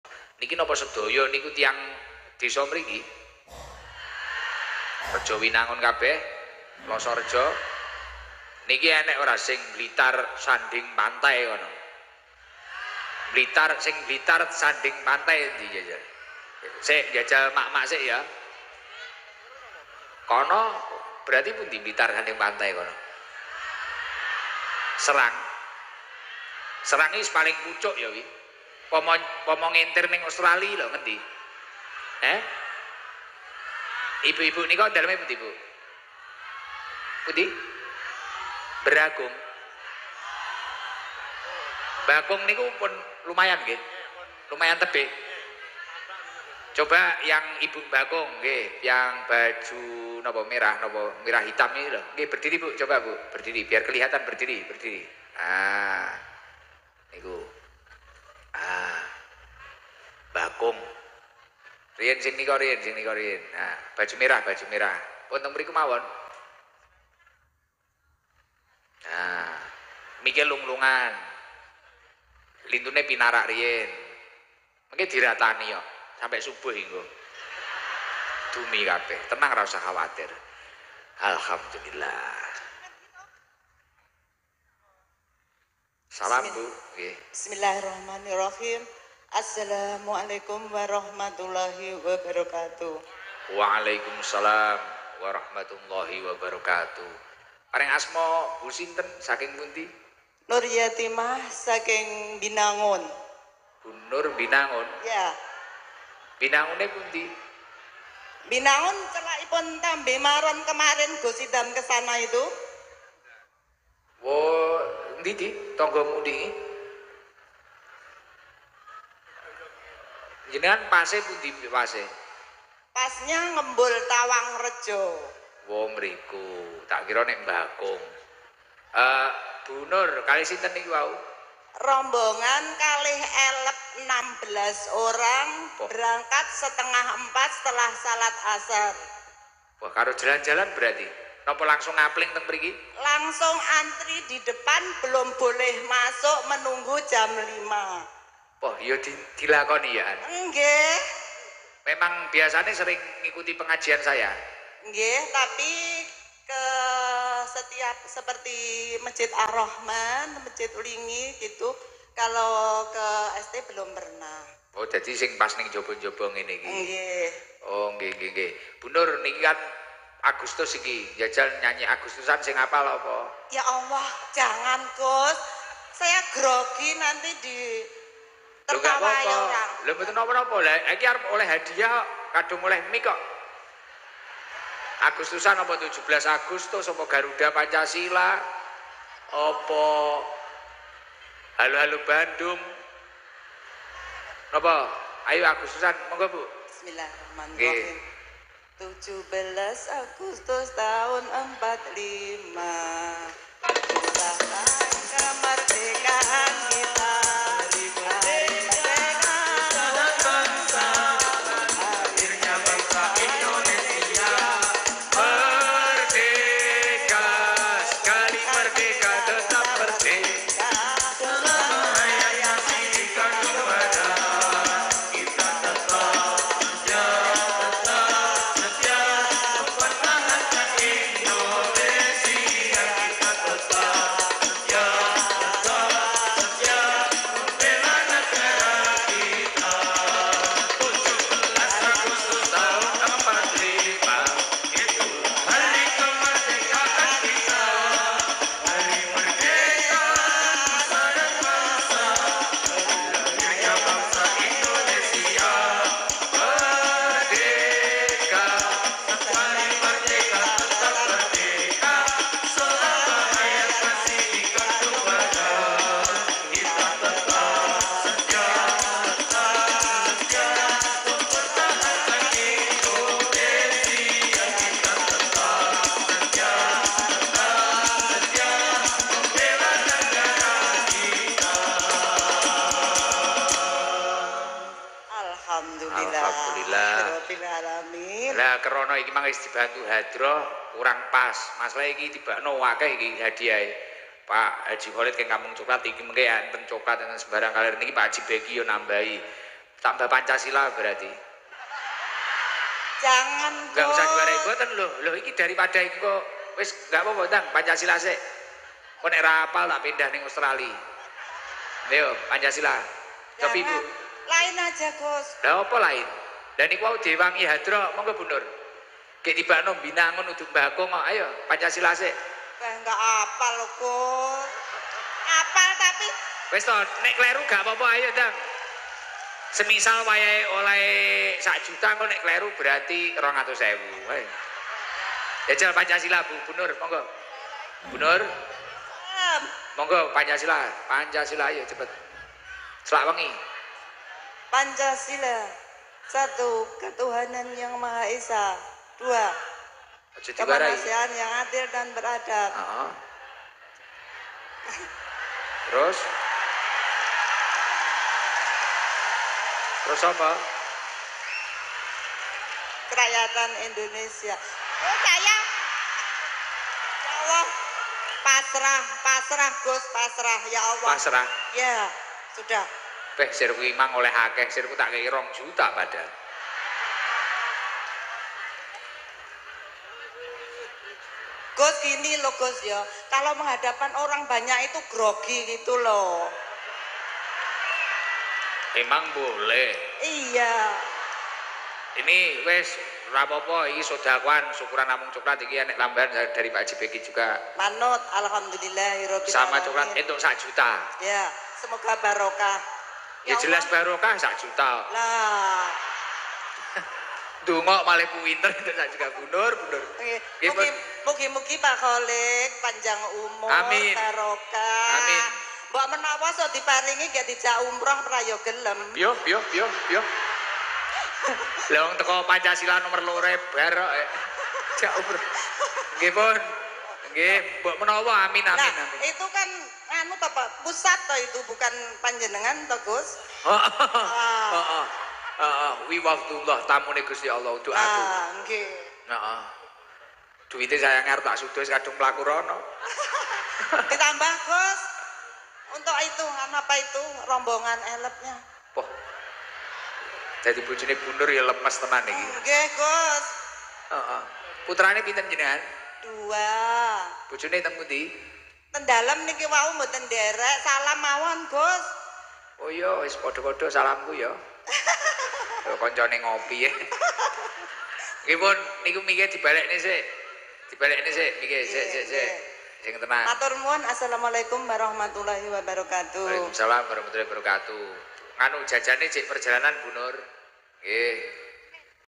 Bu nur jadi bahan tertawaan jamaah saat Gus Iqdam nyuruh baca pancasila 12 Agustus 2024
pengajian gus iqdam,ngaji bareng gus iqdam,gus iqdam terbaru